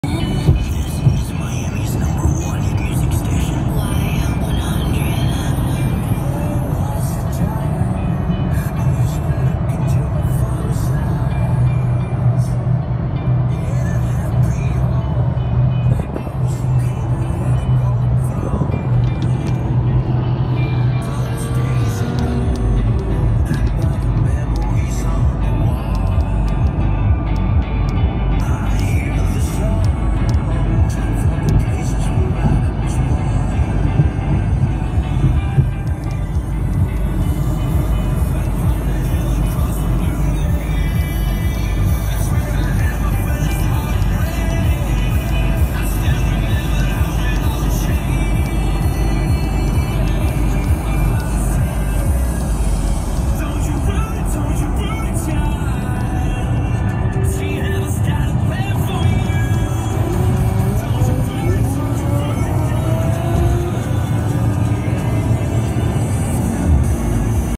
Drמg race ⏱ Ferrari vs. Lamborghini
Red Lamborghini vs. White Ferrari 🎐 on hiקhway